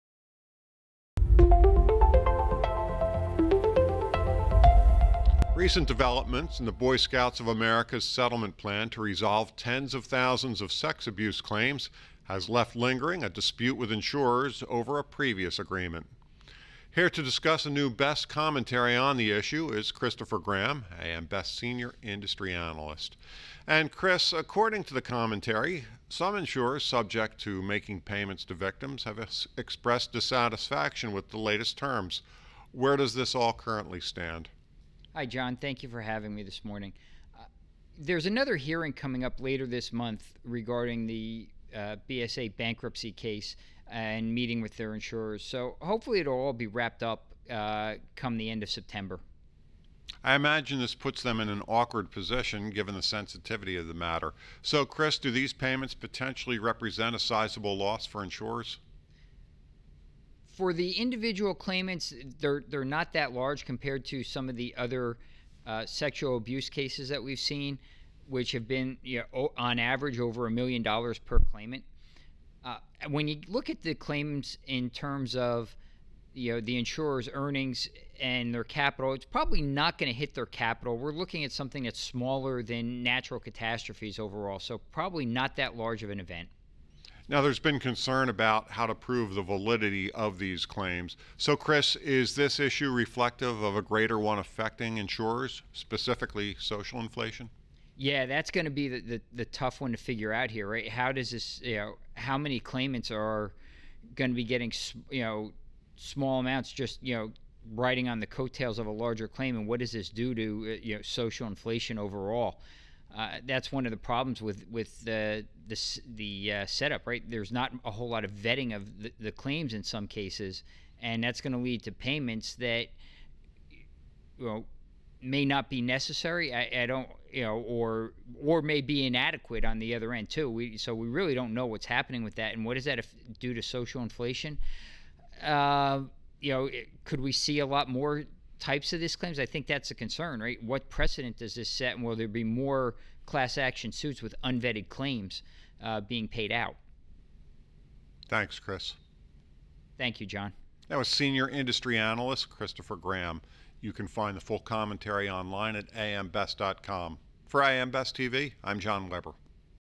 AM Best: Boy Scouts of America Legal Dispute Poses Obstacles for Insurers
Insurers with exposure may have no choice but to challenge the validity of some claims, even though they recognize that victims deserve compensation, said Christopher Graham, senior industry analyst, AM Best (Sept. 20, 2021).